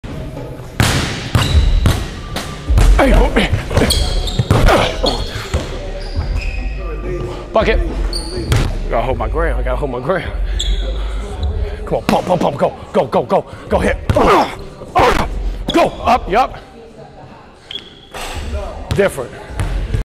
Chicago Bulls Pat Williams in training
Game reps with Chicago Bulls, Patrick Williams with Tyler Relph.